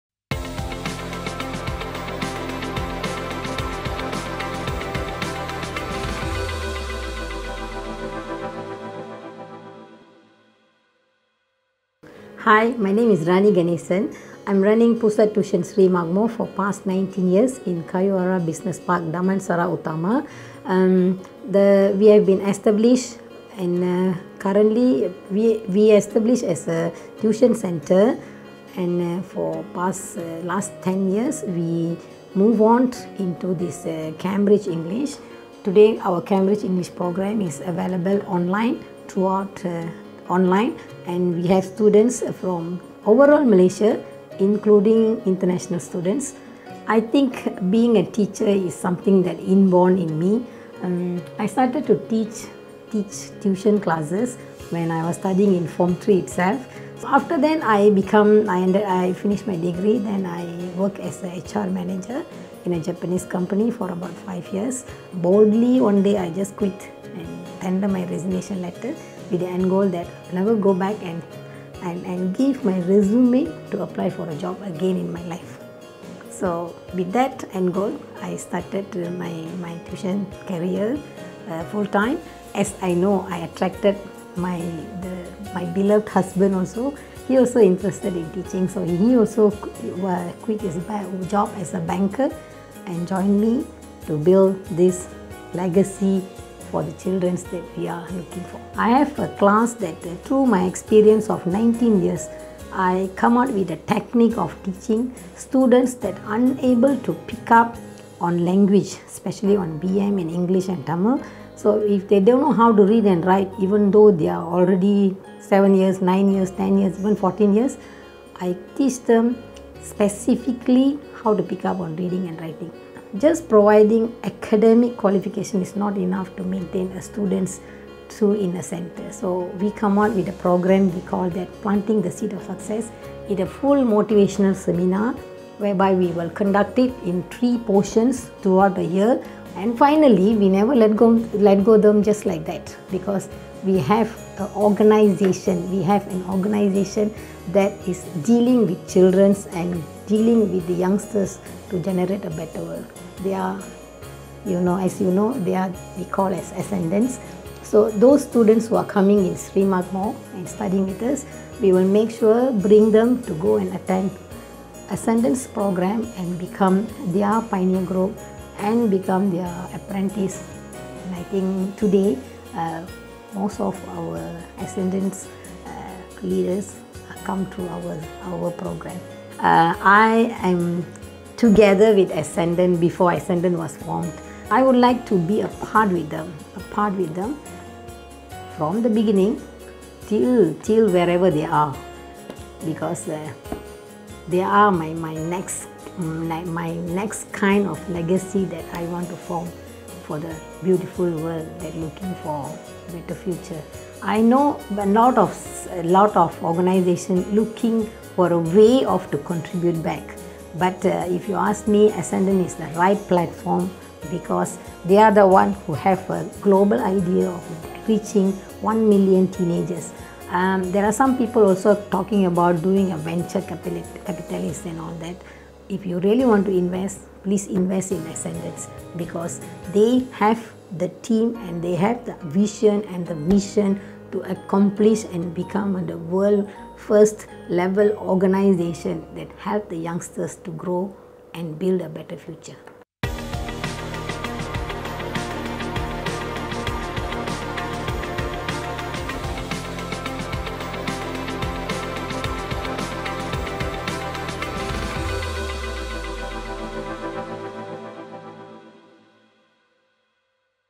Ascendance's Corporate Partner - Pusat Tuisyen Sri Makmur
Rani Ganesan is the Founder of Pusat Tuisyen Sri Makmur that has been running the past 19 years.

PTSM provides tuition classes from standard 1 to SPM as well as Cambridge English classes. Thank you for supporting Ascendance in our mission to impact OneMillionTeens and Building The Future together.